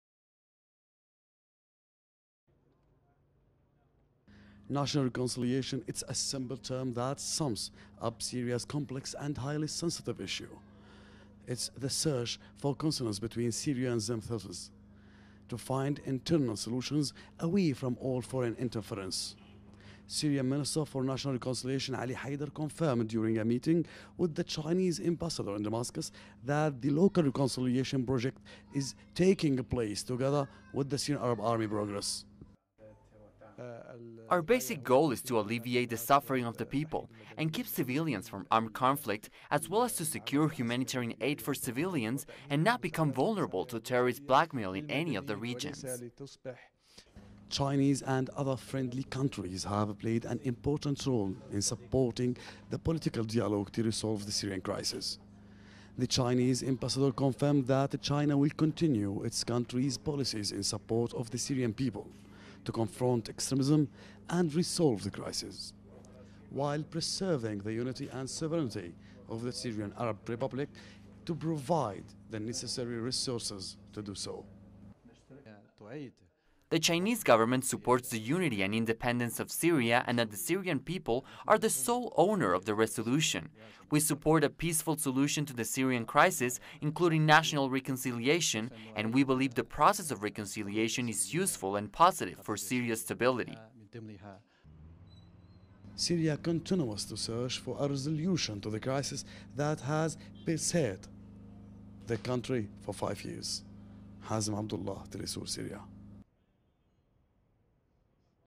Pushing for National Reconciliation in Syria
National reconciliation... It's a simple term that sums up the complex and highly sensitive issue in Syria. It is the search for consensus between Syrians themselves, to find Internal solutions, without foreign intervention. Hazem Abdullah has the latest.